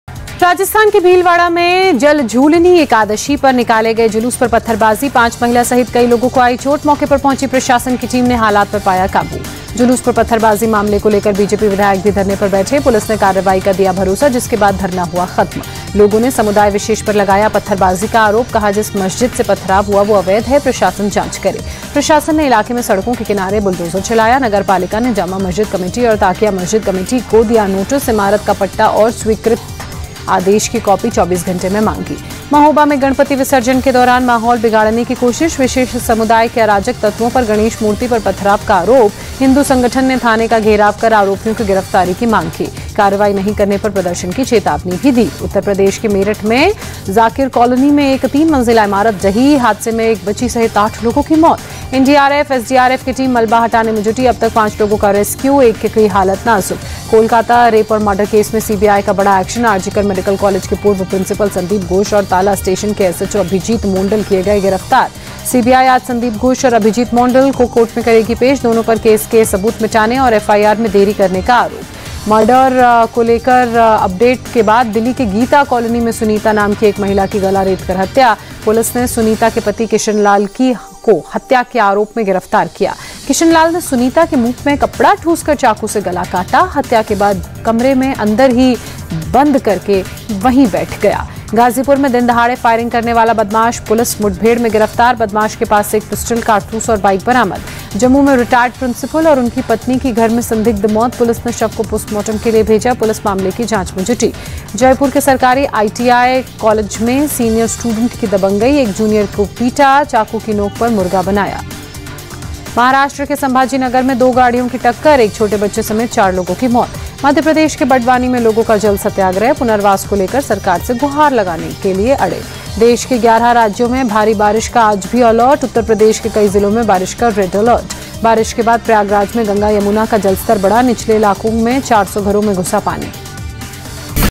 Rajasthan Bhilwara News | BJP Vidhayak | Mahoba Ganesh Mahotsav | Uttar Pradesh | Speed News
Rajasthan Bhilwara News: भीलवाड़ा से सटे शाहपुरा जिले के जहाजपुर में जलझूलनी के जुलूस पर हुए पथराव के बाद वहां तनावपूर्ण शांति बनी हुई है.

Rajasthan Bhilwara News: After stone-pelting on the Jaljhulni procession in Jahazpur, Shahpura district, adjacent to Bhilwara, a tense peace has prevailed in the area.